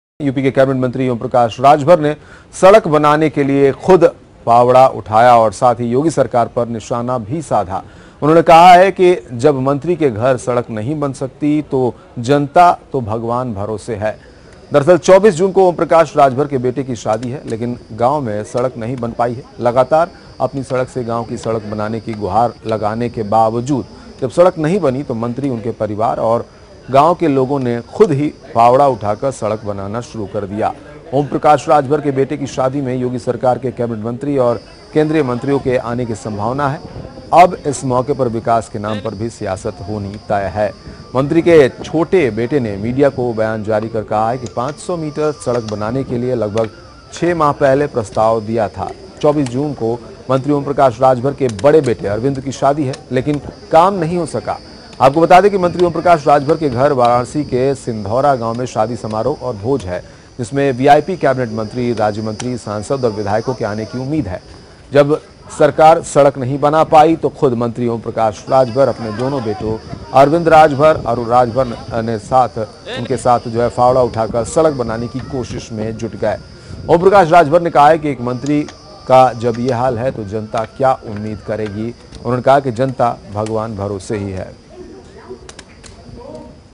मंत्रीजी बने मजदूर
योगी के मंत्री ने उठाया फावड़ा,UP के कैबिनेट मंत्री ओमप्रकाश राजभर ने सड़क बनाने के लिए फावड़ा उठा लिया ,राजभर ने कहा जब मंत्री के घर सड़क नहीं बन सकती तो जनता का क्या ??